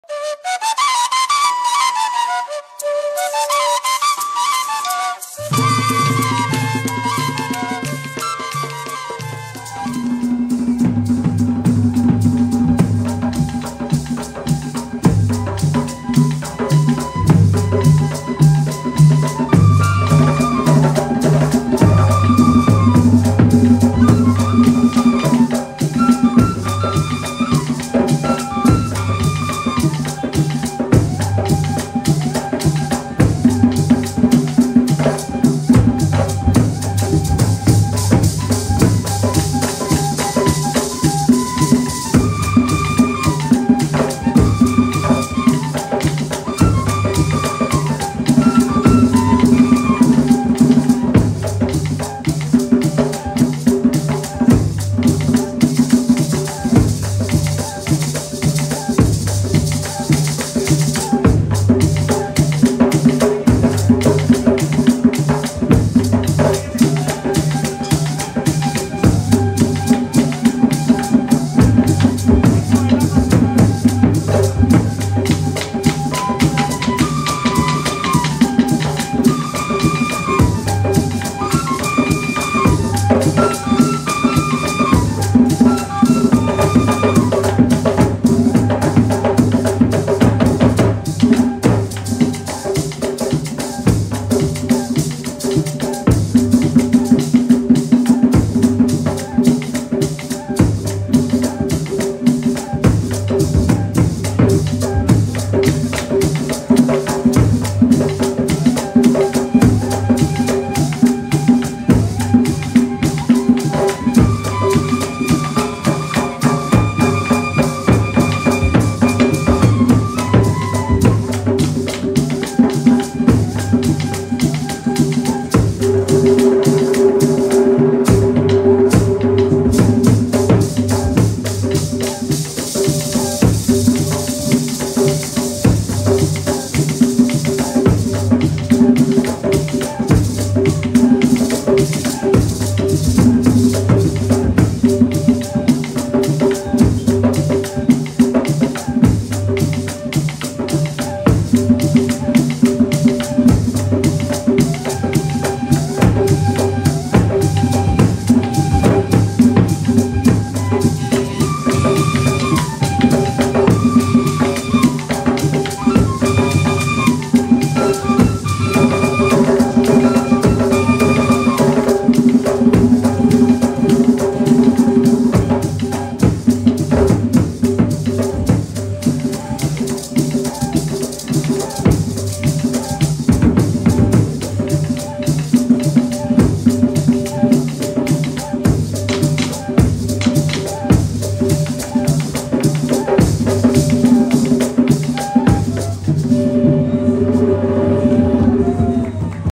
Festival de Cuadros Vivos de la Cultura Zenú, Tuchín - 2024
La "Performance"  del cuadro vivo es una representación actoral pública, efímera, de una escena detenida en el tiempo, sobre un  suceso, una alegoría o un motivo, generalmente de carácter religioso, moral, histórico y a veces satírico.